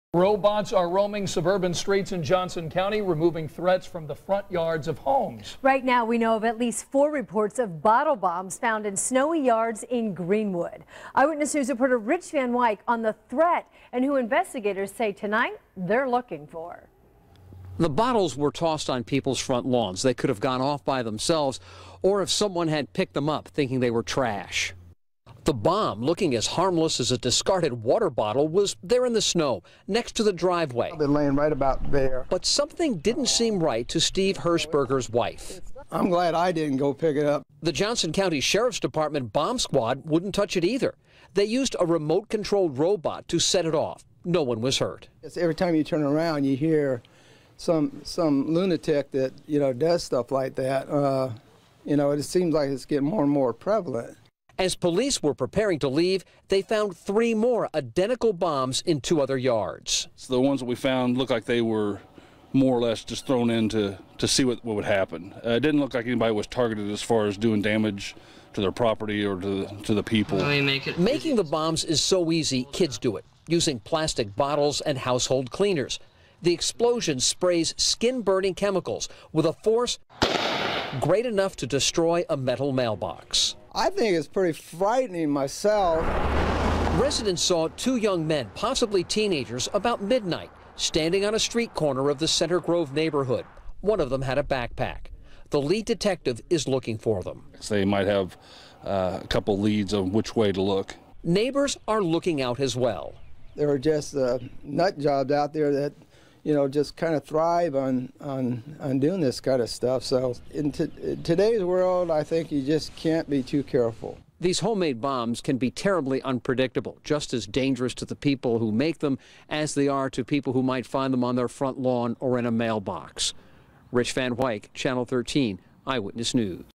Johnson County bottle bombs
Robots are roaming suburban streets in Johnson County removing threats from the front yards of homes.